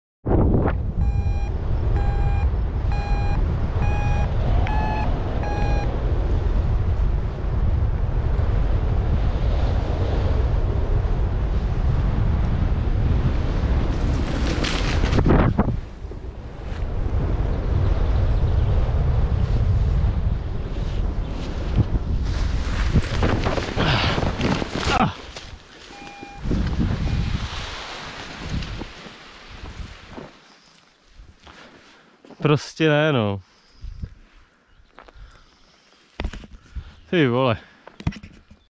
Paragliding fail / win, emergency landing through trees
Staying too long close to the hill finding a lift can end up like this. I was lucky, but a single downwind would have put me on a tree.